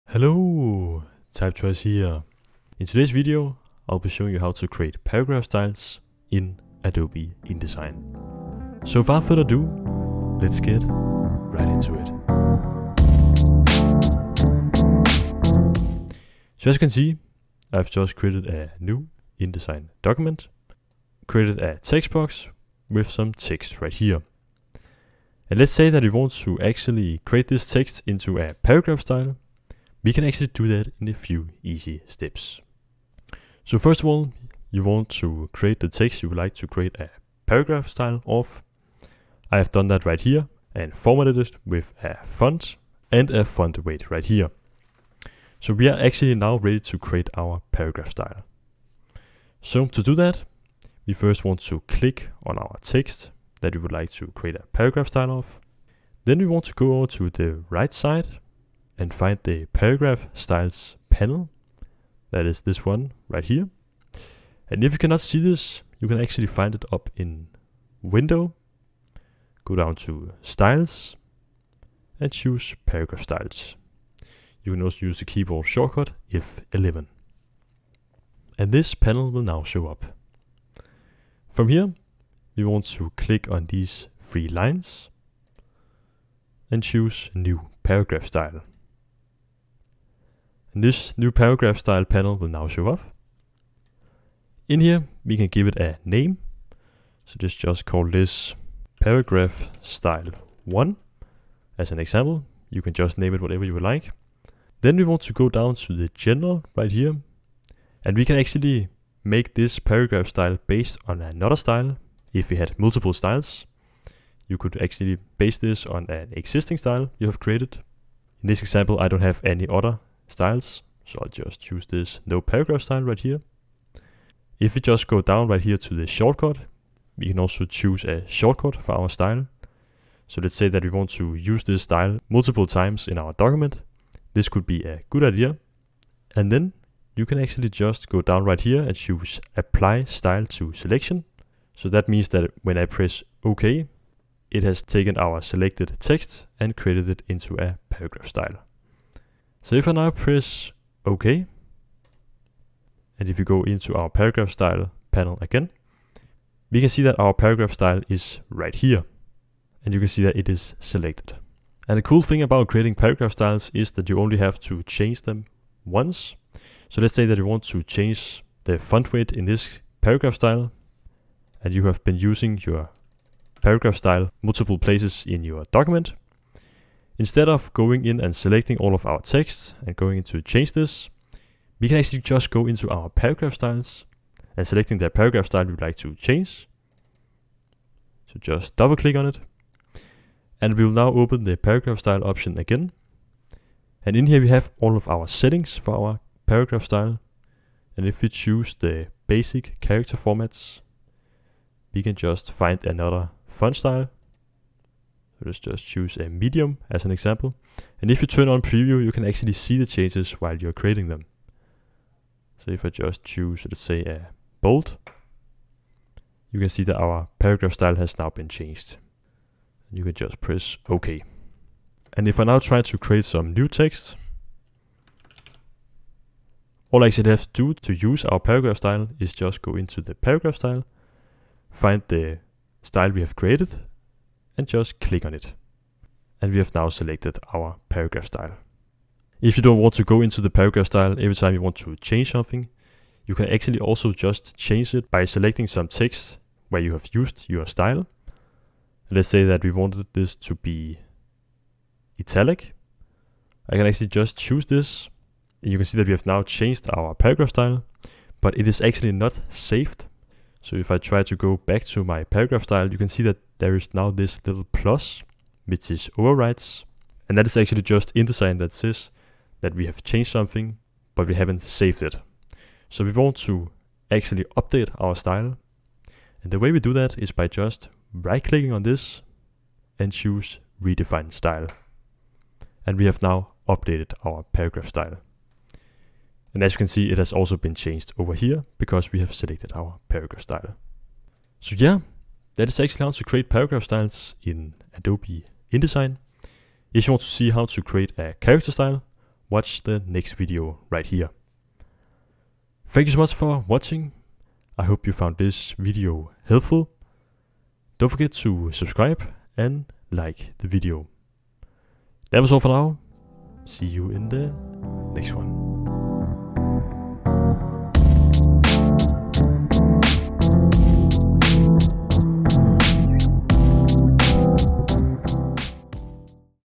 How to Create Paragraph Styles In Adobe InDesign
In today's video, I'll be showing you how to create paragraph styles in Adobe InDesign